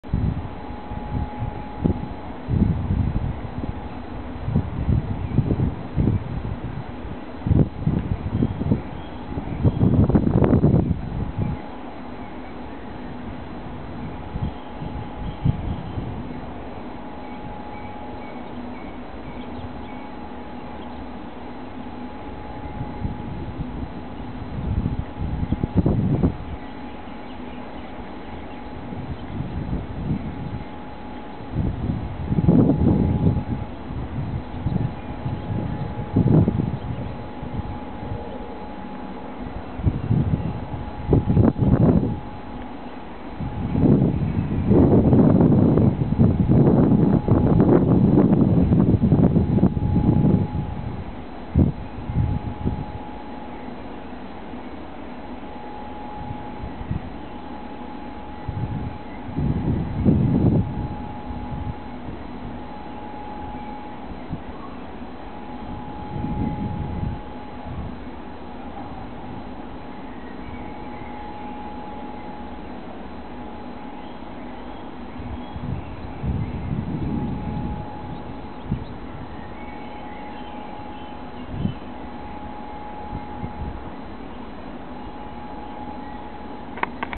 Fox in the Paddocks
The vixin that raised the cubs last year with a pigeon she caught in the paddocks on the farm